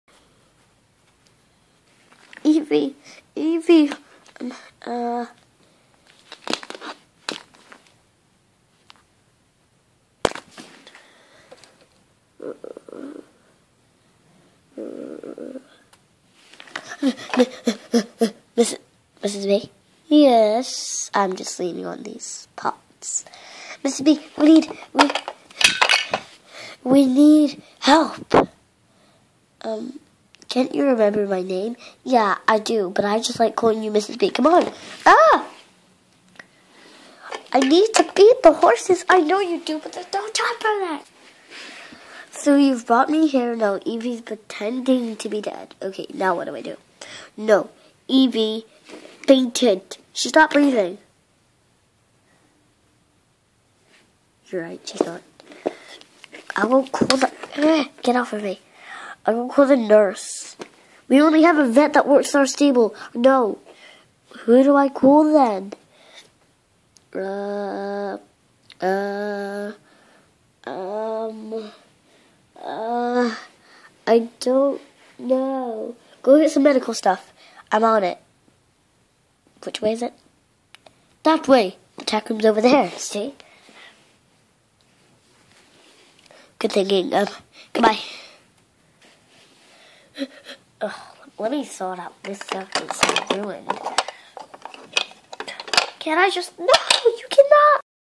we will miss you evie
this is a video off all of evies friends loosing all hope evie will still be with them